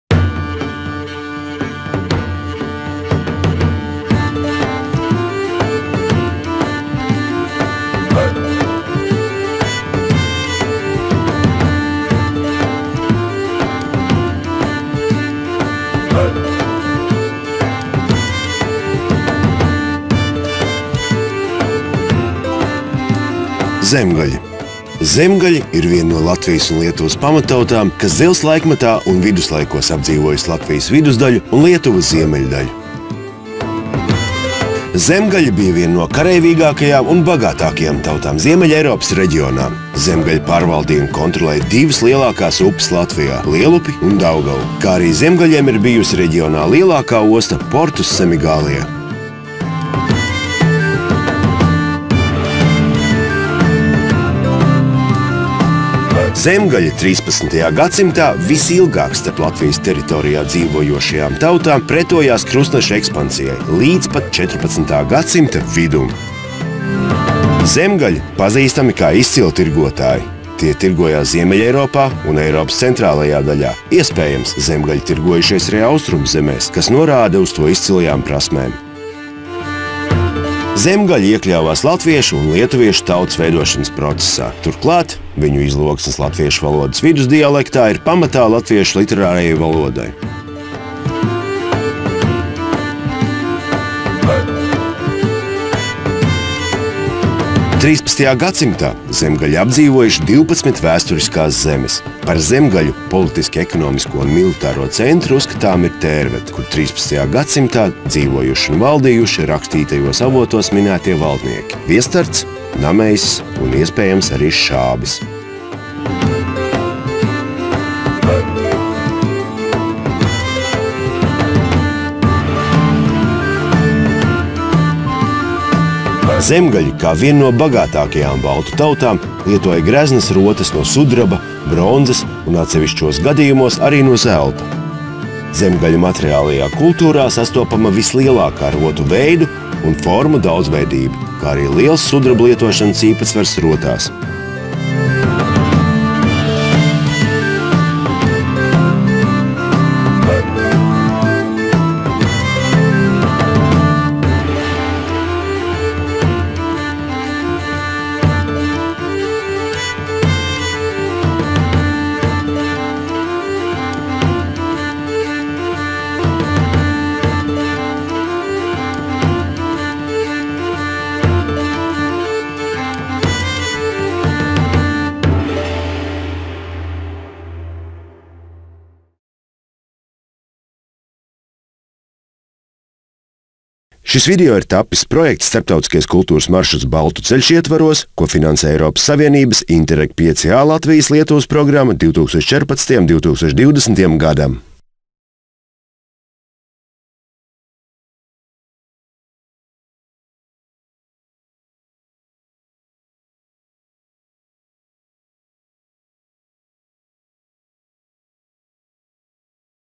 Balts' Road - The majesty of ancient Semigallians
Zemgaļi  un sēļi – senas baltu tautas, vienas no Latvijas un Lietuvas pamattautām, kas dzelzs laikmetā un viduslaikos apdzīvojusi Latvijas vidusdaļu (Zemgales līdzenumu), Lietuvas ziemeļdaļu, Latvijas dienvidaustrumdaļu un Lietuvas ziemeļdaļu.